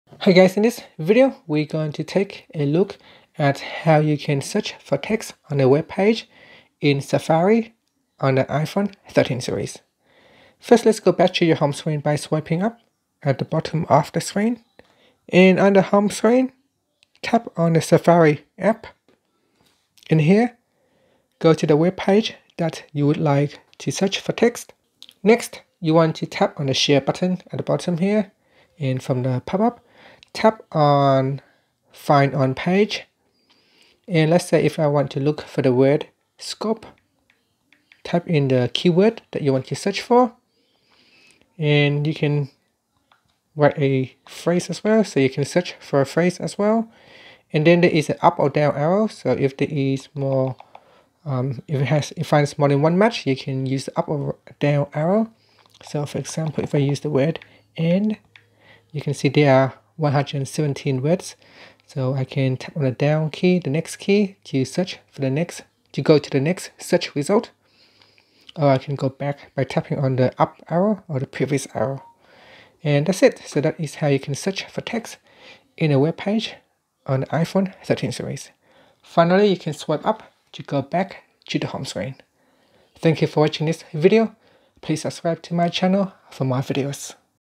iPhone 13/13 Pro: How to Search For Text In a Webpage In Safari
Learn how you can search for text in a webpage in Safari on the iPhone 13 / iPhone 13 Pro/Mini.